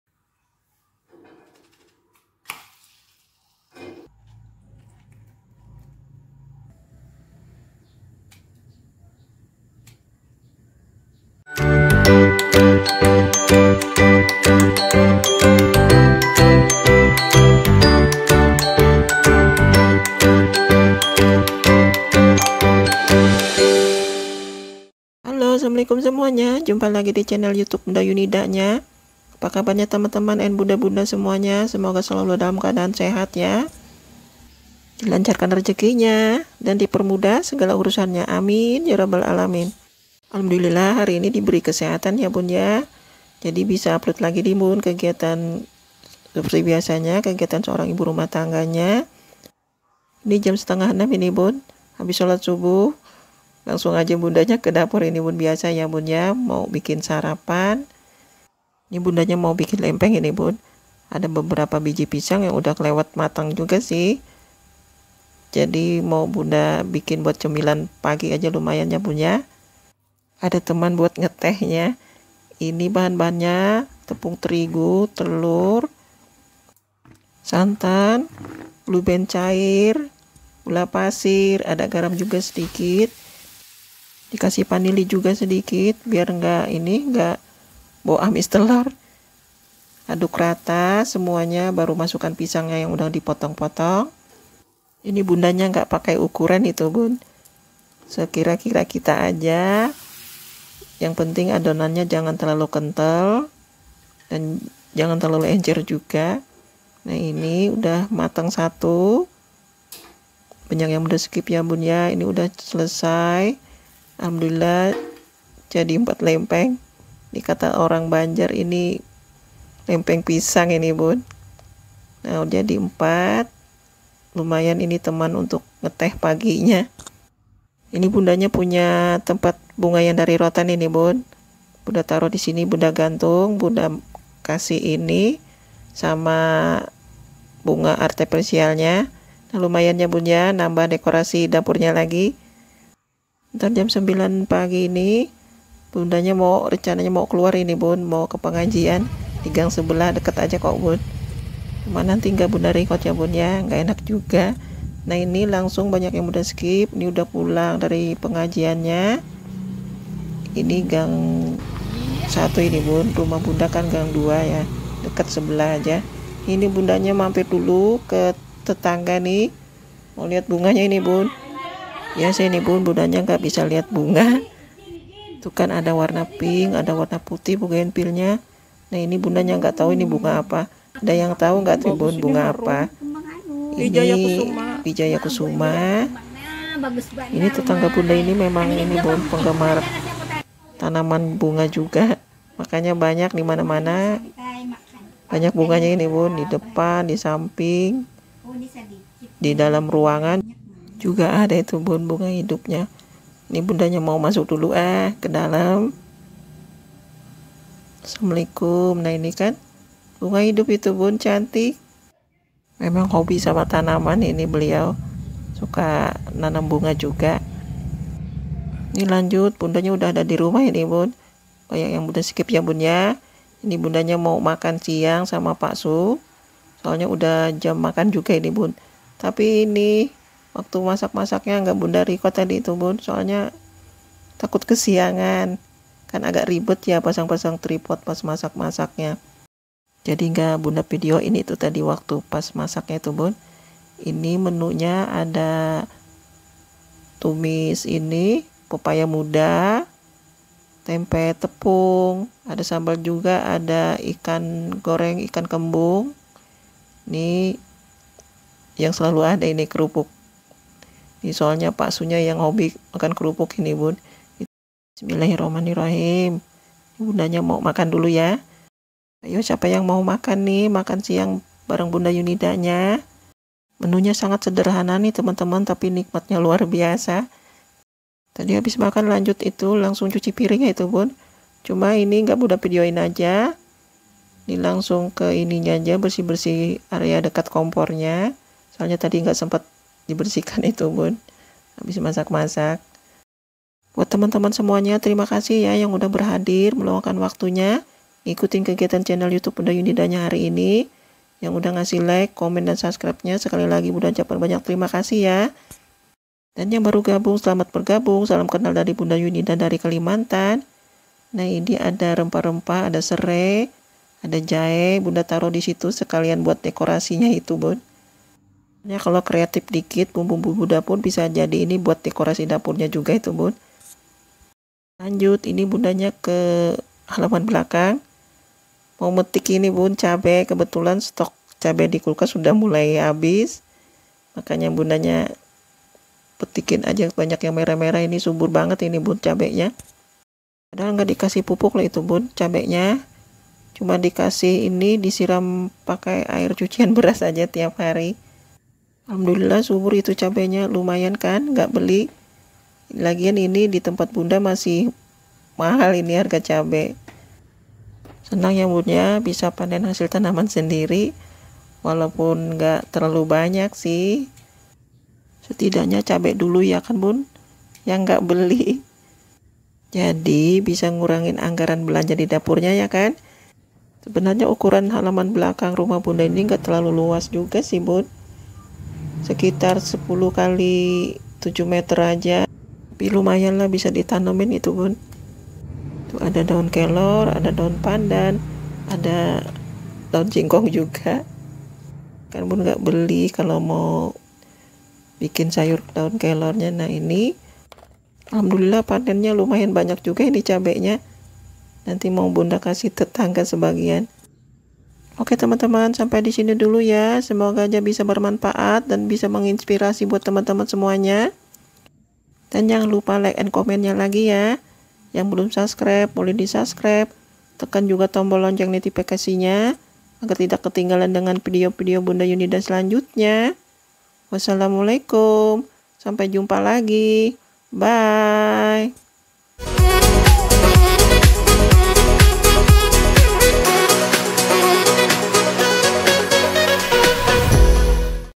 Vlog IRT ll IRT Produktif ll Awal kegiatan di pagi hari ll bikin lempeng pisang
Jumpa lg di chanel you tube Asni yunida
chanel tentang kegiatan ibu rmh tangga,awal kegiatanku di pagi hari ,bikin kue lempeng pisang,dan nambah decorasi dapur sederhana,mohon dukungannya ya teman2 dgn lake,comend,subcr nya,terima kasih..